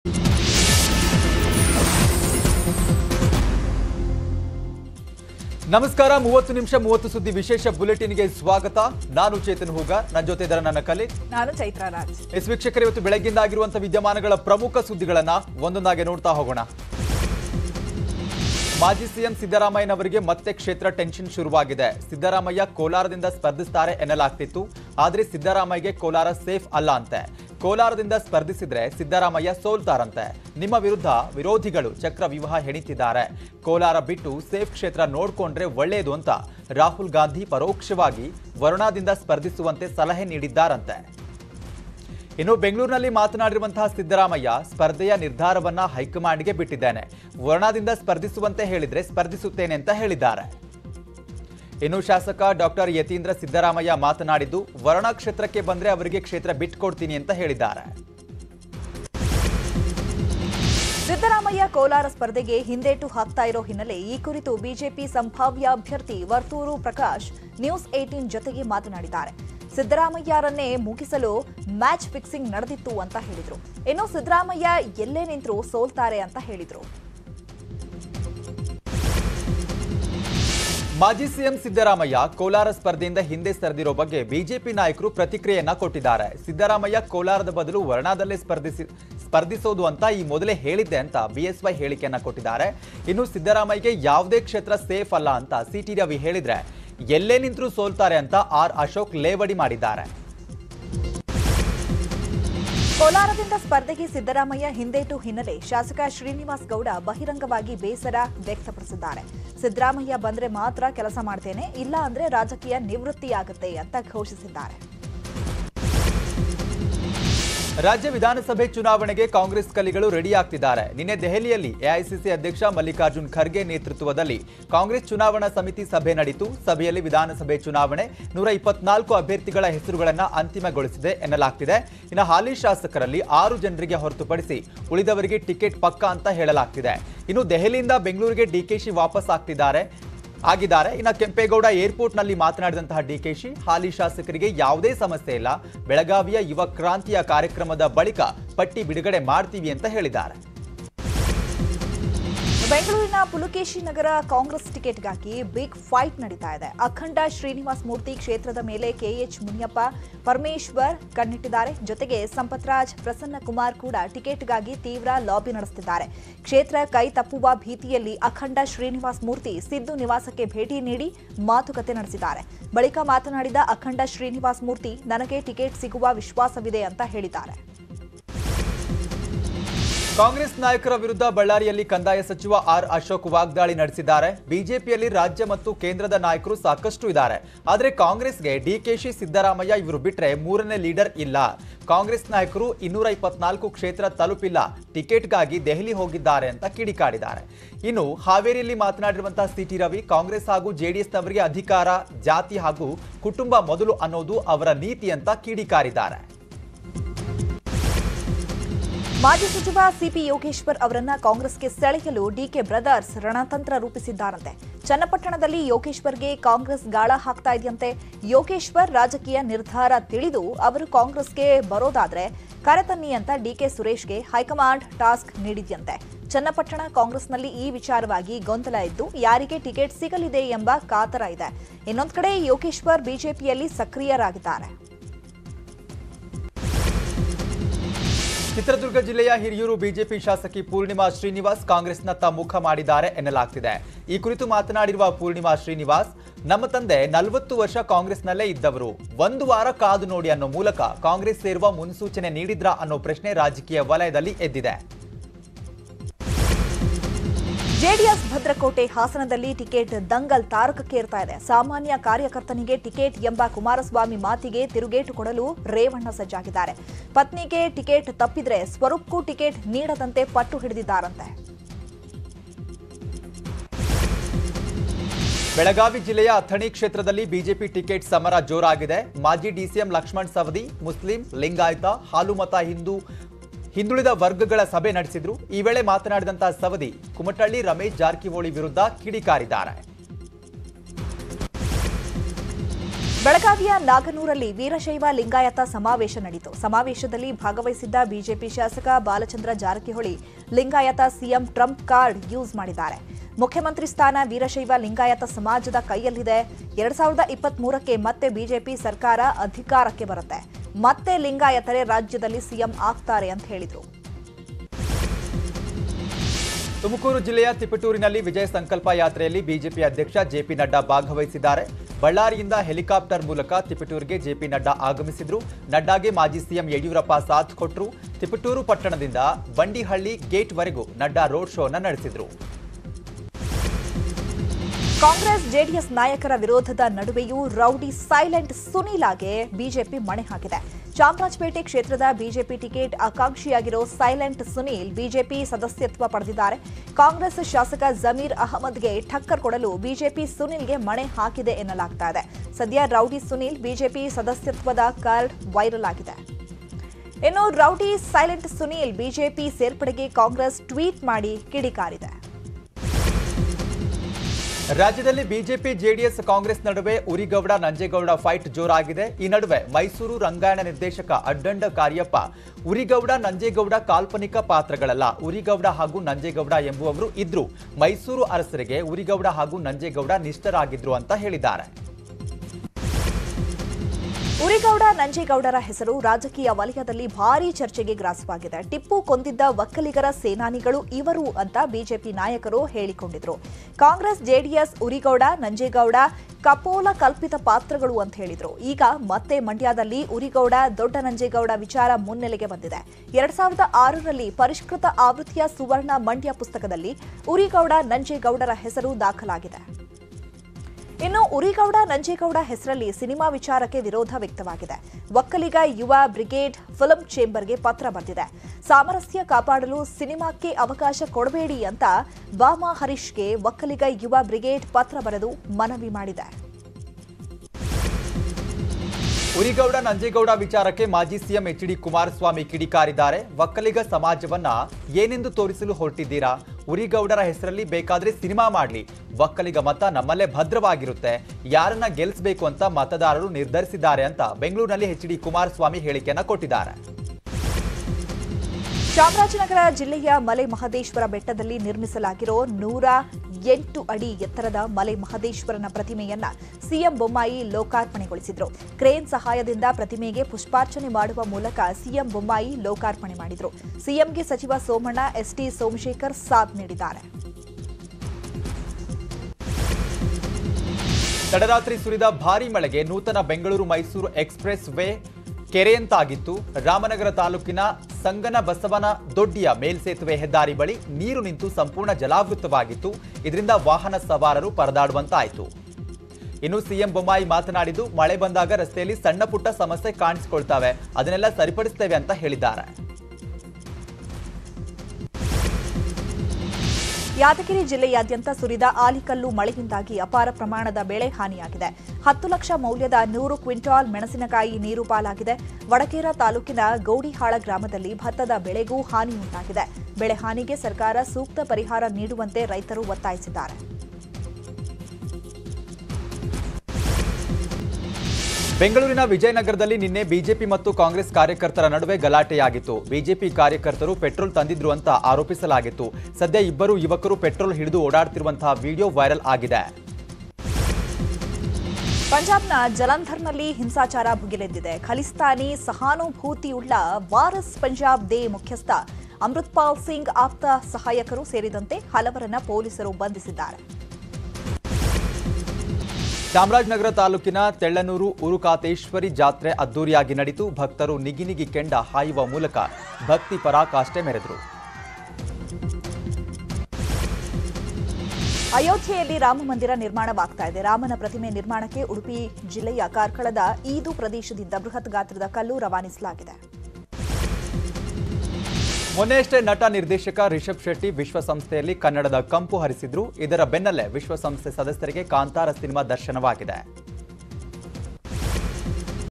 Yadgir Chill Famers Problem | 10 ಲಕ್ಷ ಮೌಲ್ಯದ 100 ಕ್ವಿಂಟಾಲ್ ಮೆಣಸಿನಕಾಯಿ ನೀರು
Yadgir Chill Famers Problem | ಯಾದಗಿರಿ ಜಿಲ್ಲಾದ್ಯಂತ ಸುರಿದ ಆಲಿಕಲ್ಲು ಮಳೆಯಿಂದಾಗಿ ಅಪಾರ ಪ್ರಮಾಣದ ಬೆಳೆ ಹಾನಿಯಾಗಿದೆ. 10 ಲಕ್ಷ ಮೌಲ್ಯದ 100 ಕ್ವಿಂಟಾಲ್ ಮೆಣಸಿನಕಾಯಿ ನೀರು ಪಾಲಾಗಿದೆ. ವಡಗೇರಾ ತಾಲೂಕಿನ ಗೋಡಿಹಾಳ ಗ್ರಾಮದಲ್ಲಿ ಭತ್ತದ ಬೆಳೆಗೂ ಹಾನಿಯುಂಟಾಗಿದೆ.